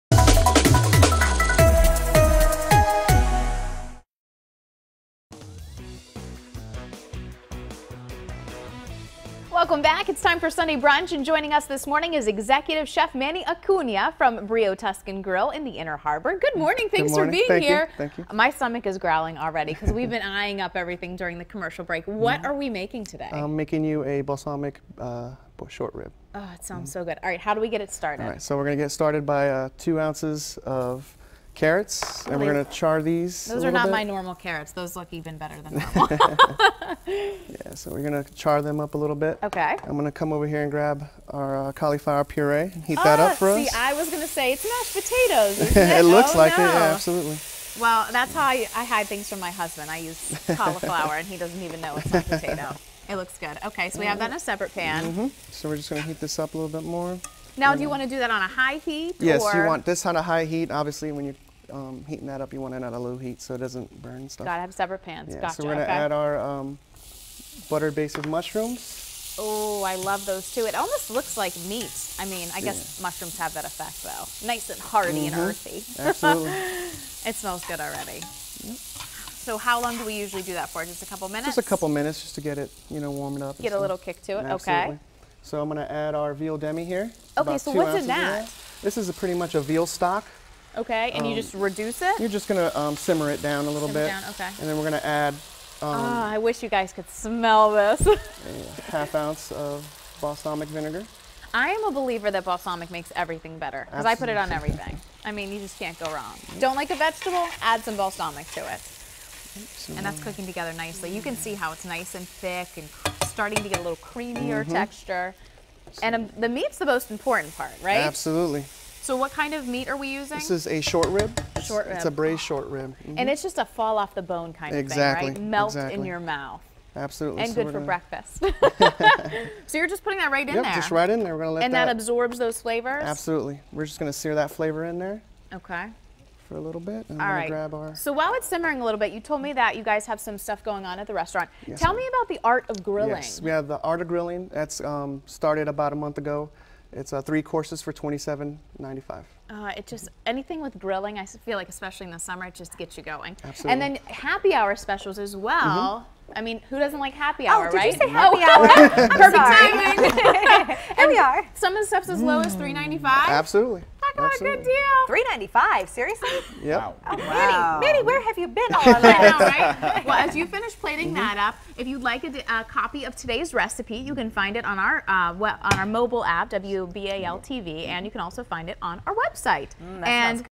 Sunday Brunch: Brio Tuscan Grille's balsamic short rib
Manny Acuna with Brio Tuscan Grille serves up his balsamic short rib.